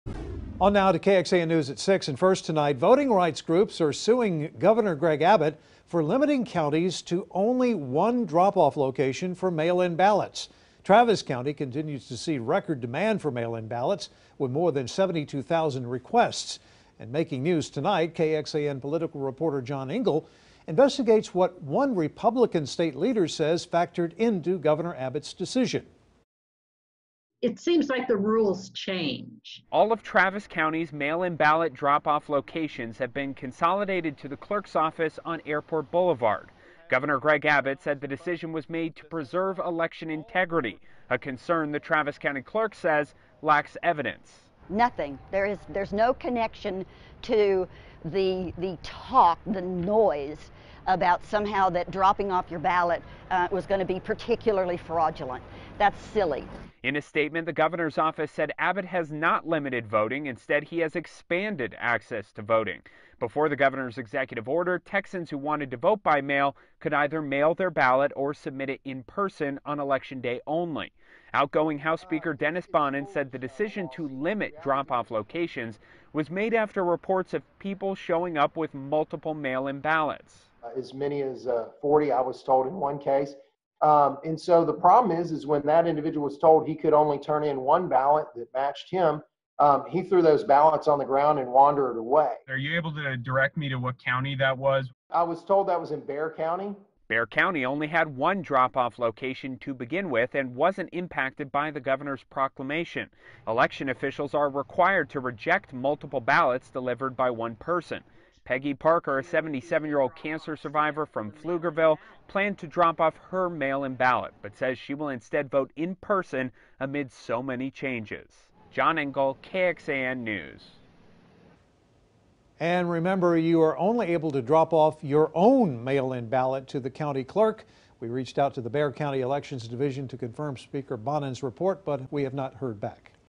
Lawsuit filed against Gov. Greg Abbott for limiting ballot drop off locations
A lawsuit has been filed against Gov. Greg Abbott and county election officials in Texas after the governor issued a proclamation that limits each county to one ballot drop-off location.